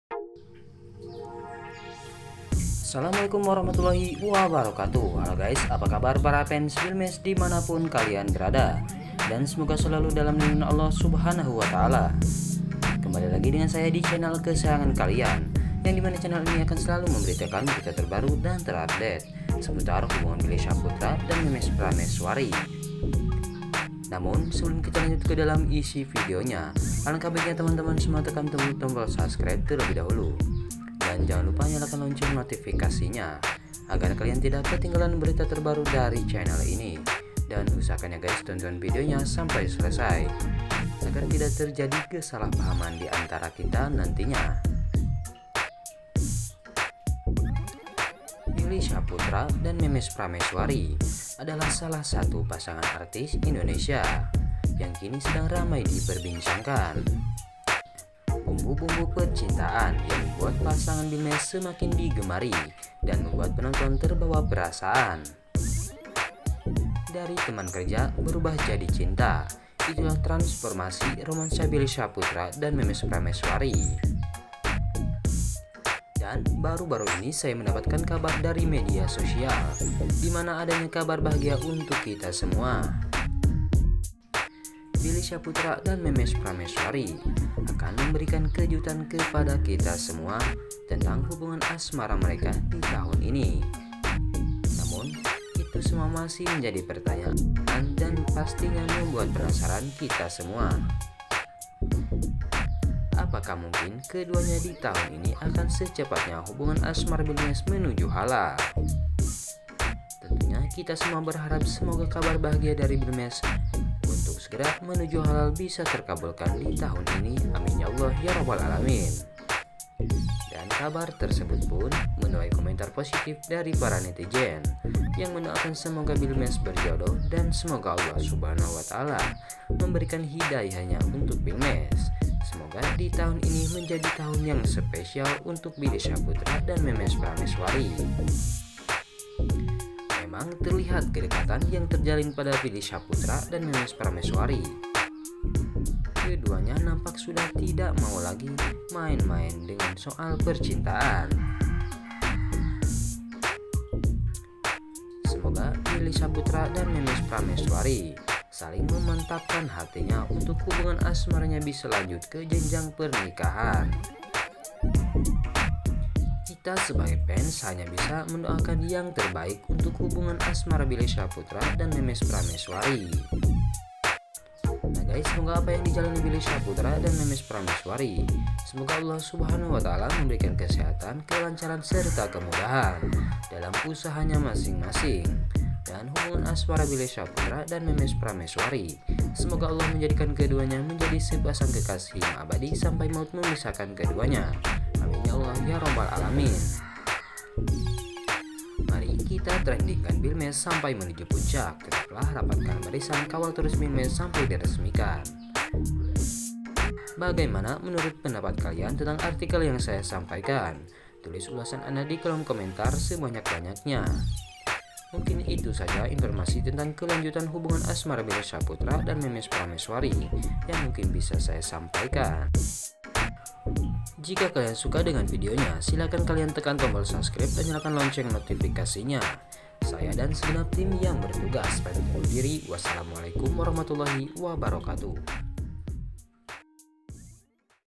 BILLY SYAHPUTRA DAN MEMES PRAMESWARI Ada Yang Special Dari Bilmes Setelah lebaran Idul Adha
Assalamualaikum.
Selamat datang di channel NEW BOLLY .
Untuk teman teman yang baru bergabung,